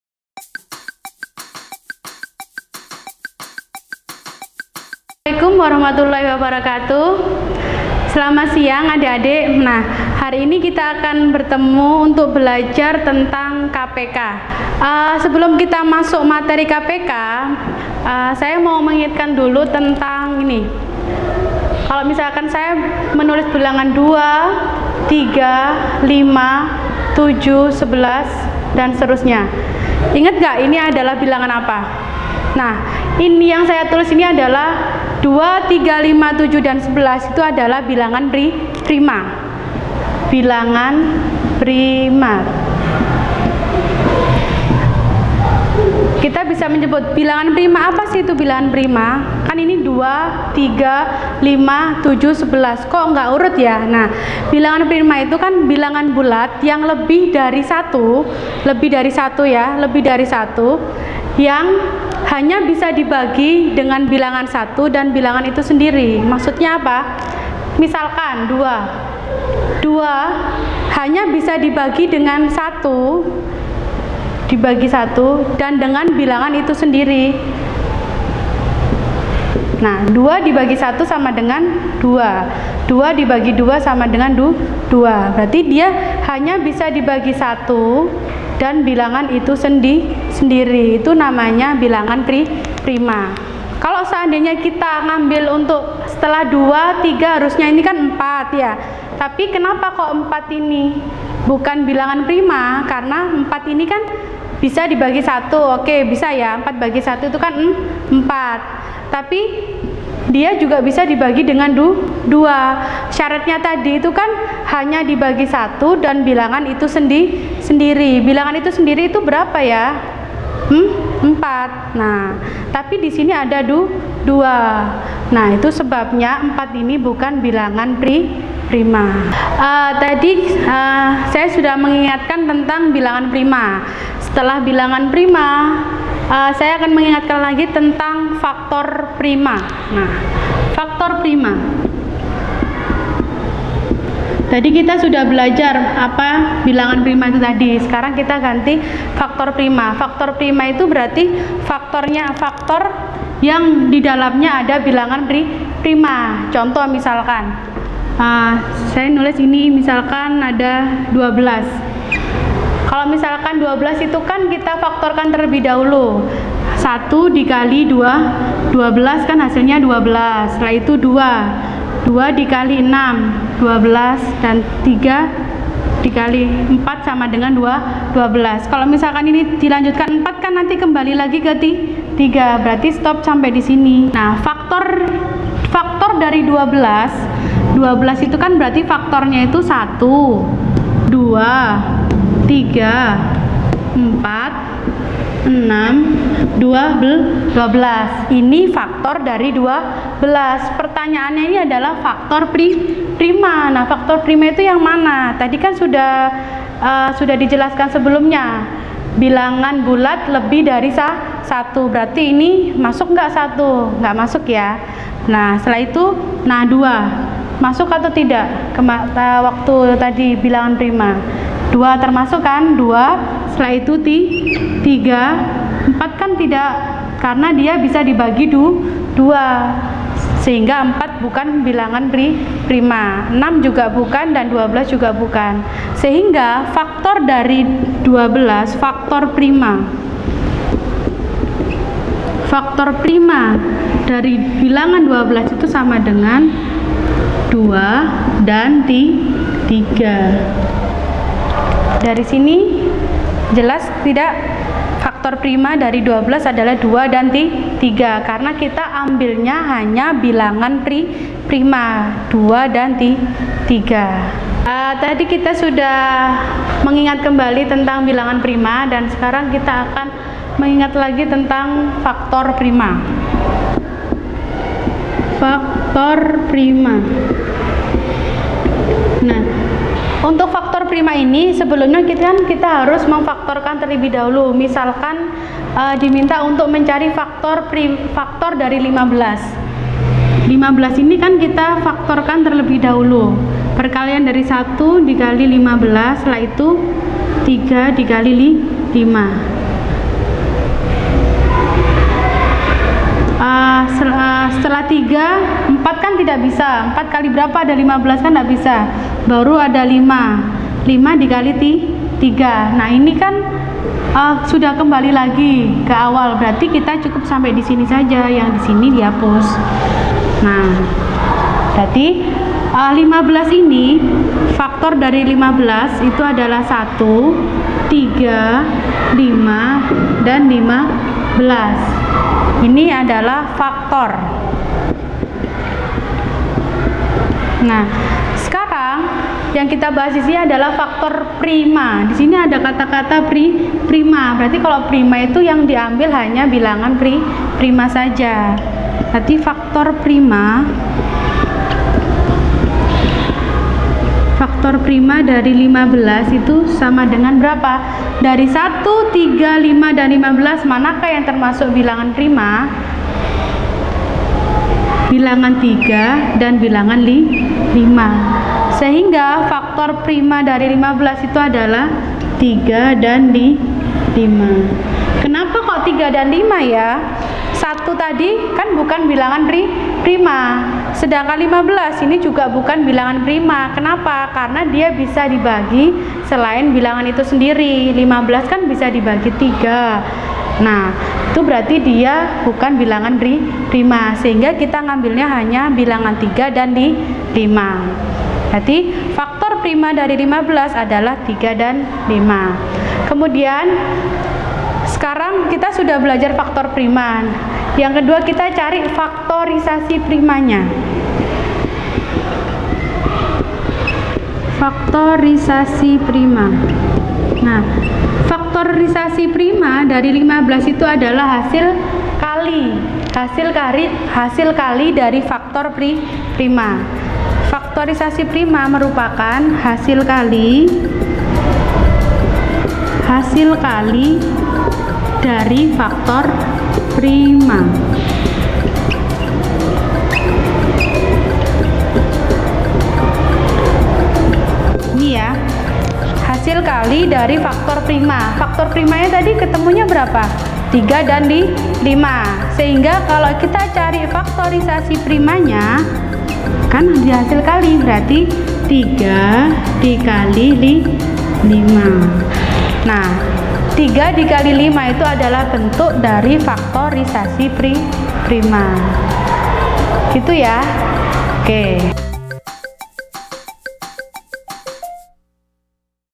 Kelas 06 - Matematika - Faktorisasi Prima | Video Pelajaran Sekolah K13
Bilangan prima adalah bilangan bulat yang lebih dari satu yang hanya bisa dibagi dengan bilangan 1 dan bilangan itu sendiri. Lalu apa bedanya dengan faktorisasi prima??

Yuk pelajari selengkapnya tentang Faktorisasi Prima bersama Ibu Junita Widyawati, S.Pd dari SD Mardi Putera.